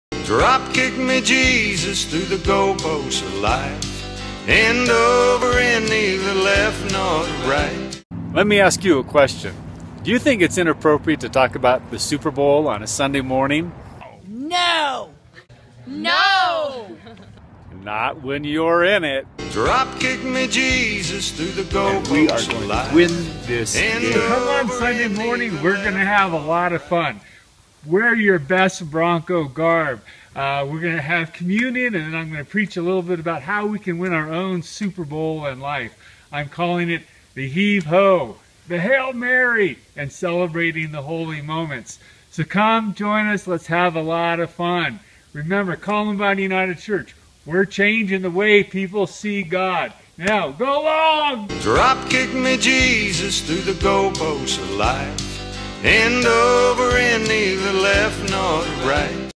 Super Bowl Sunday!
Wahoo! The Bronco's are in the Super Bowl. This Sunday is the big game! Let's fill the house! We're going to celebrate communion and I have a special sermon all set to make sure we win the Super Bowl!